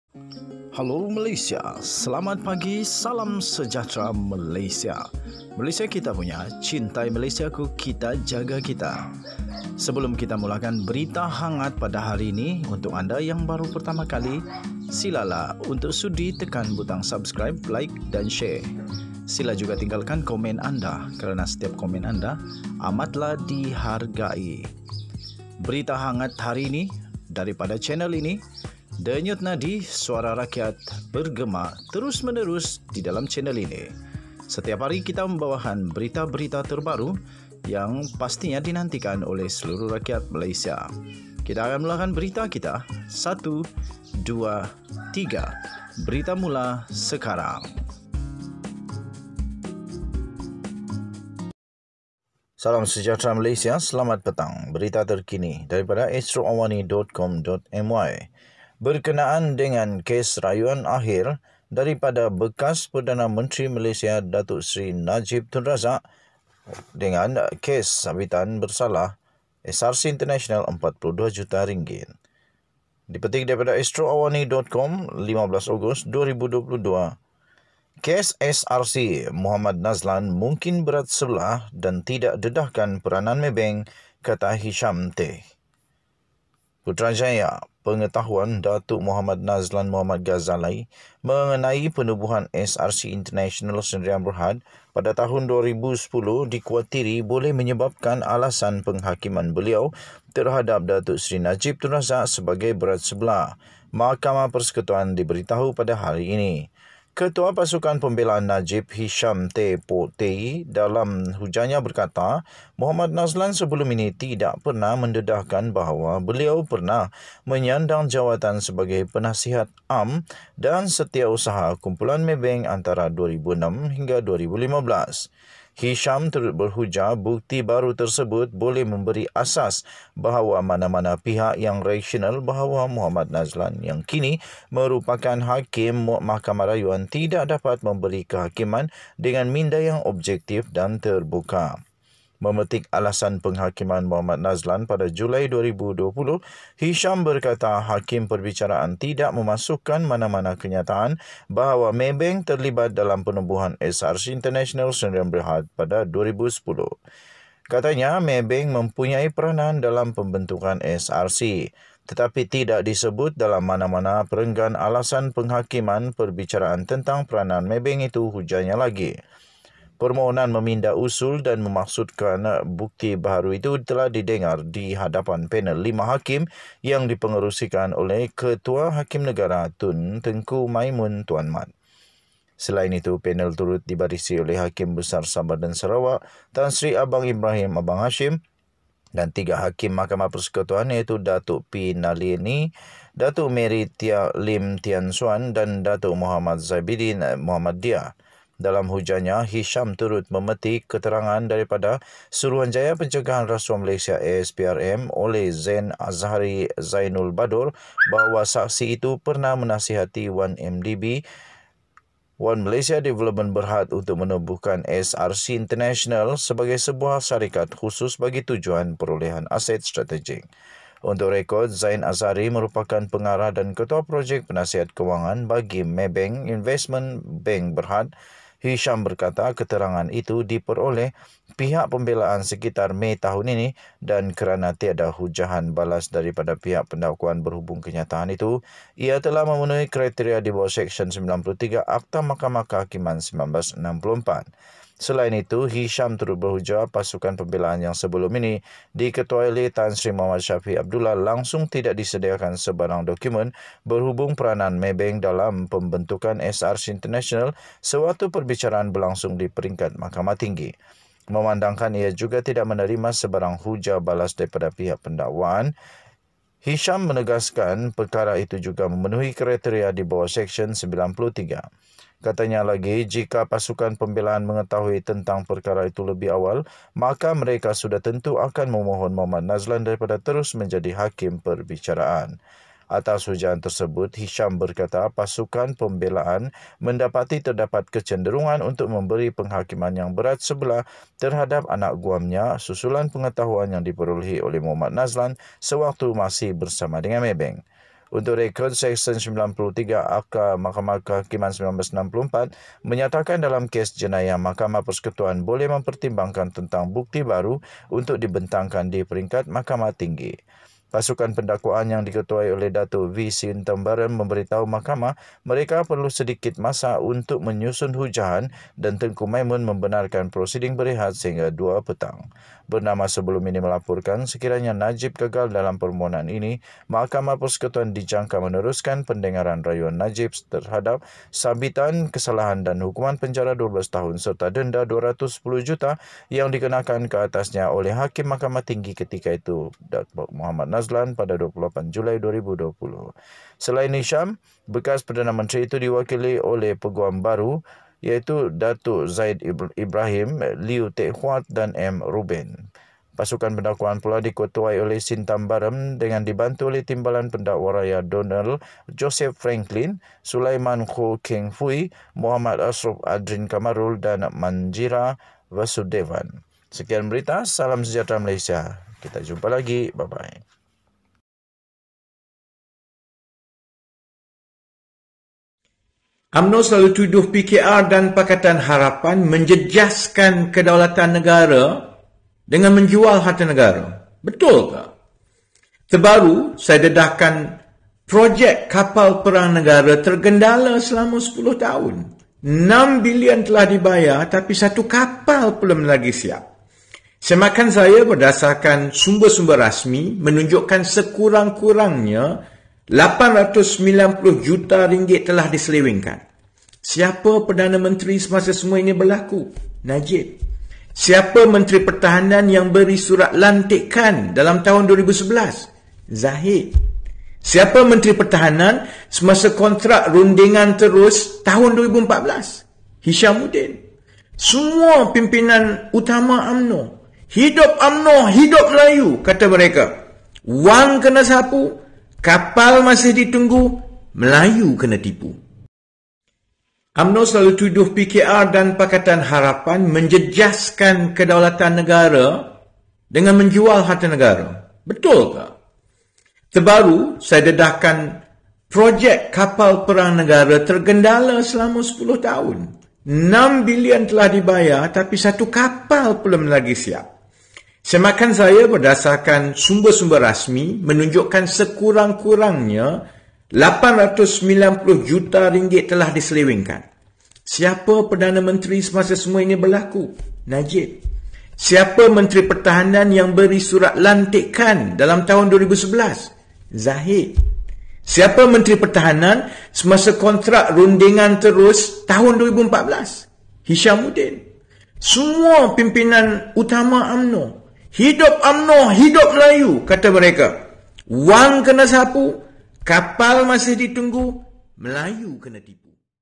[LANGSUNG] KEPUTUSAN TERKINI DI MAHKAMAH PERSEKUTUAN RAYUAN TERAKHIR NAJIB RAZAK SRC RM42 JUTA
INILAH KEPUTUSAN TERKINI MAHKAMAH PERSEKUTUAN NAJIB RAZAK